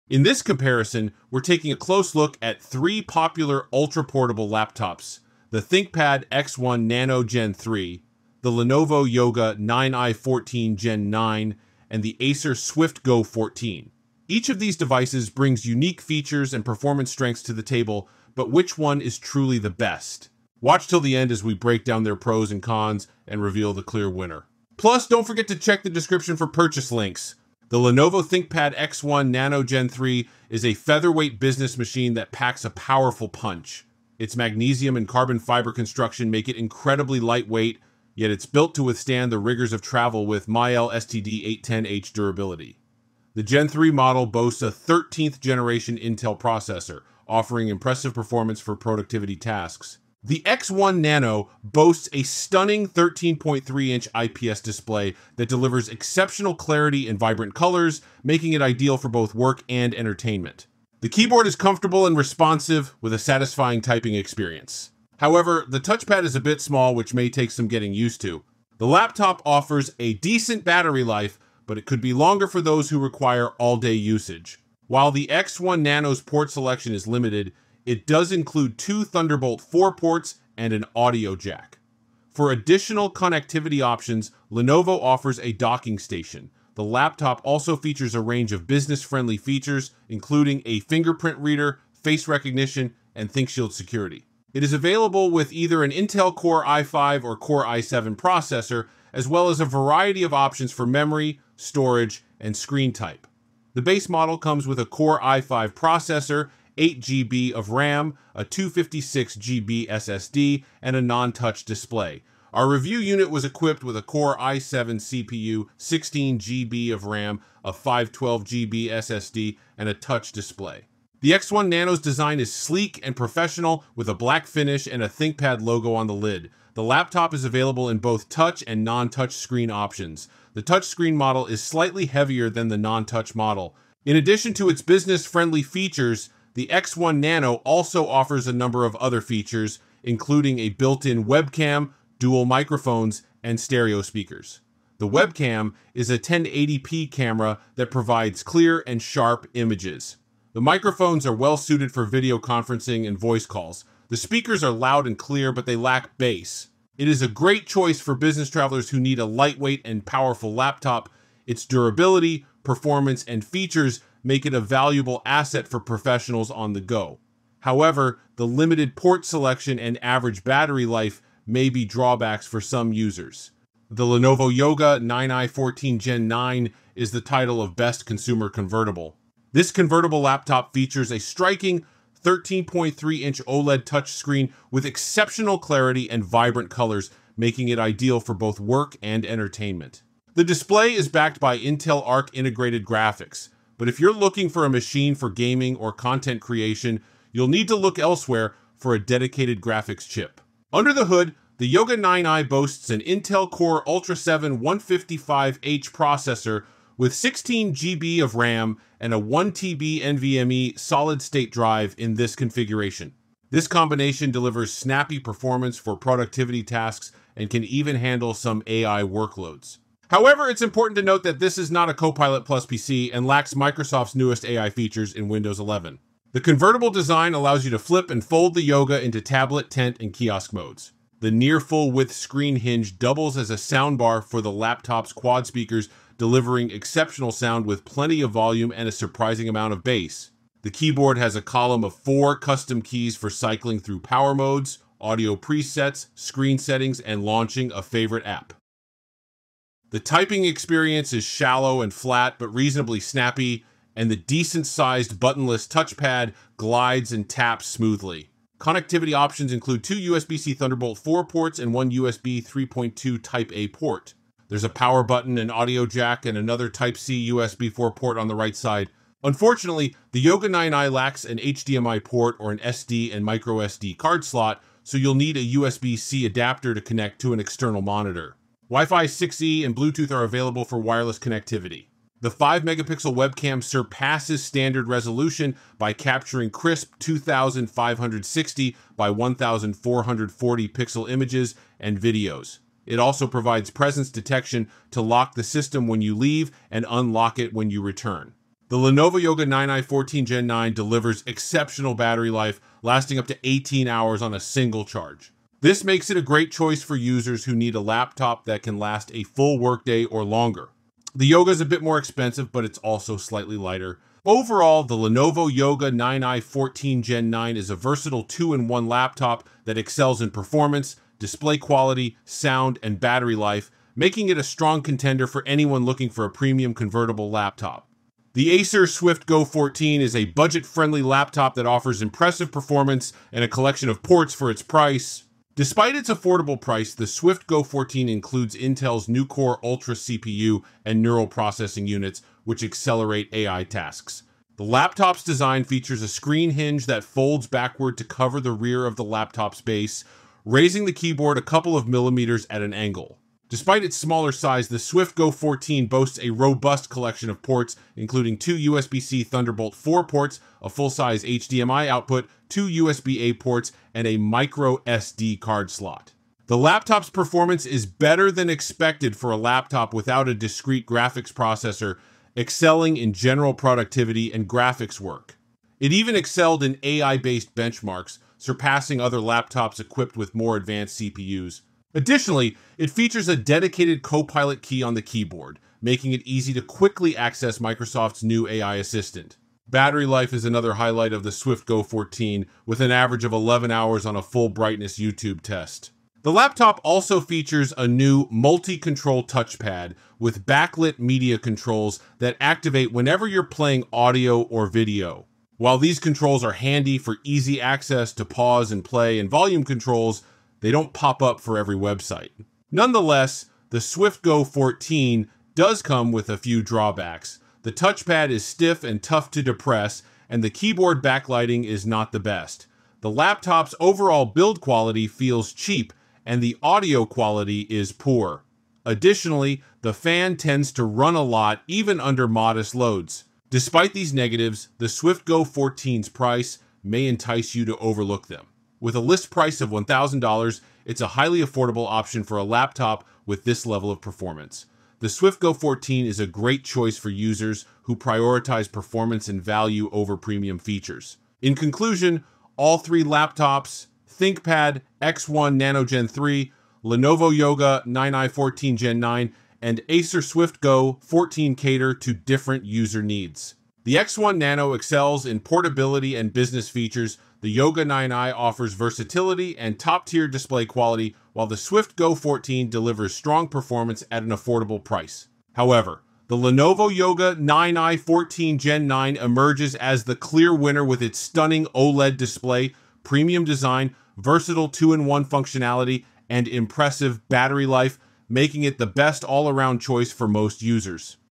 ThinkPad X1 Nano Gen 3 vs Yoga 9i vs Swift Go 14: The Unexpected Champion
➜USA Links:

ThinkPad X1 Nano Gen 3
Lenovo Yoga 9i 14 Gen 9
Acer Swift Go 14

ThinkPad X1 Nano Gen 3 vs Lenovo Yoga 9i 14 Gen 9 vs Acer Swift Go 14: The Best Ultraportable Laptop for 2024 in the USA

Looking for the perfect lightweight laptop to take on the go? In this video, we compare three top contenders: the ThinkPad X1 Nano Gen 3, Lenovo Yoga 9i 14 Gen 9, and Acer Swift Go 14. We'll dive into their performance, features, and value to help you decide which one is the best fit for your needs.

Key topics covered:

Processor power: Intel Core Ultra and more
Display quality: OLED, IPS, and resolution
Battery life: All-day endurance for on-the-go professionals
Design and portability: Sleek and lightweight options

Don't miss our in-depth analysis and expert recommendations to help you choose the ideal ultraportable laptop for your 2024 needs.

Chapters:

0:00 - 0:27 - intro
0:28 - 2:53 - ThinkPad X1 Nano Gen 3
2:54 - 5:27 - Lenovo Yoga 9i 14 Gen 9
5:28 - 7:31 - Acer Swift Go 14
7:32 - 8:11 - conclusion